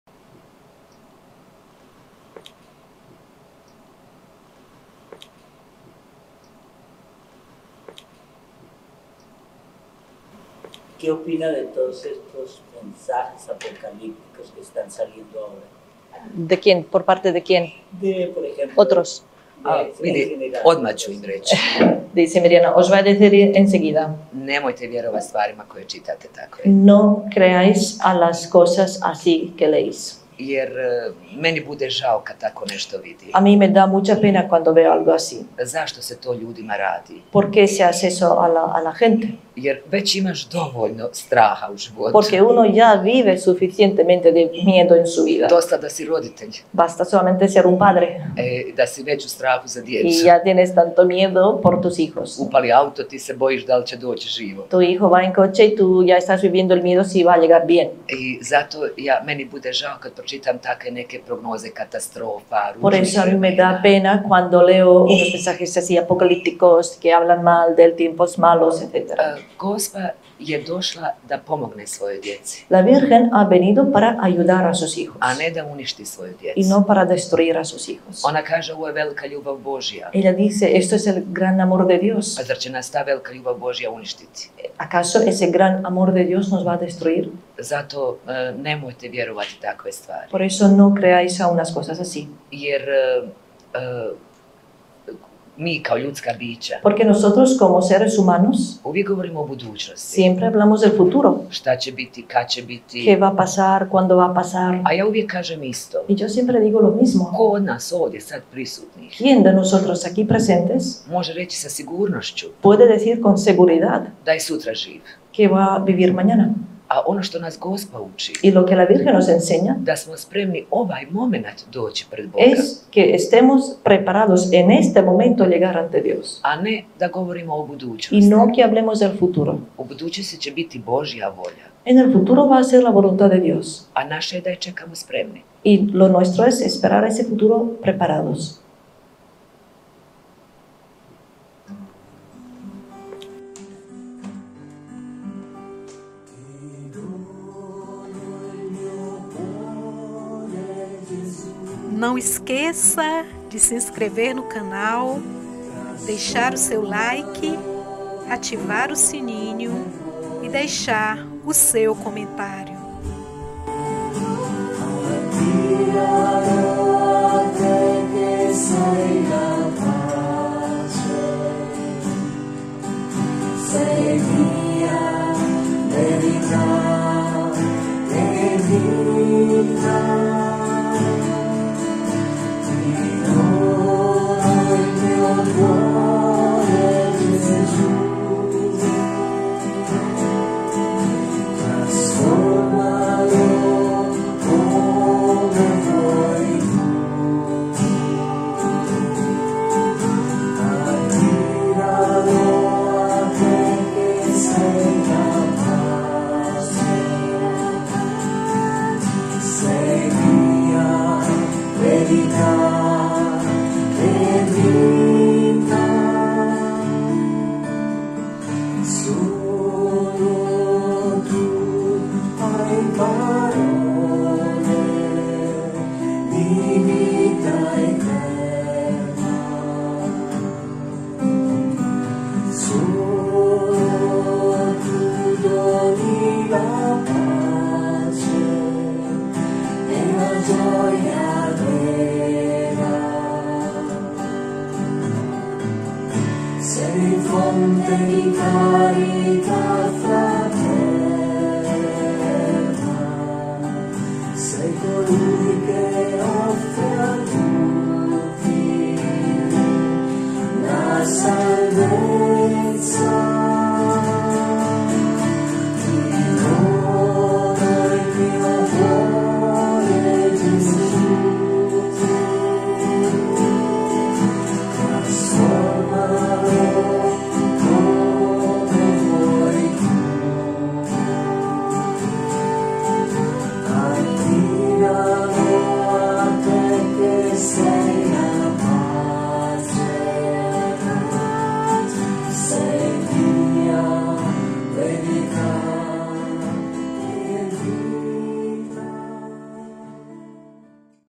MIRJANA SOLDO - MENSAGENS APOCALÍPTICAS- MEDJUGORJE- OUTUBRO 2023
Quantas mensagens vocês já receberam esses últimos meses falando do fim do mundo, do fim dos tempos , dos três dias de trevas e tantas outras com teor apocalíptico?
- Vocês acreditam nessas mensagens?
- Vocês tem medo dessas mensagens?
-De que fala as mensagens que você tem recebido atualmente ?